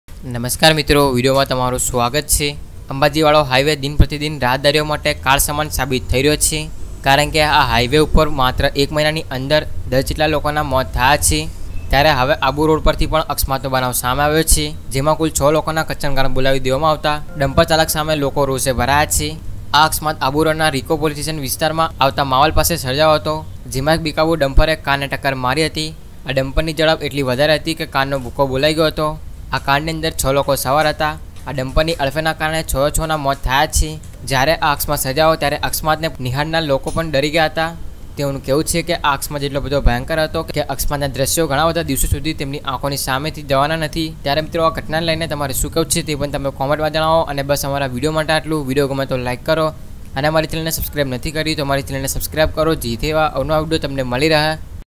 ટ્રકે અંબાજી જતા 6 લોકોને છુંદી નાખ્યા ambaji
ટ્રકે અંબાજી જતા પરિવારના 6 લોકોને છુંદી નાખ્યા ambaji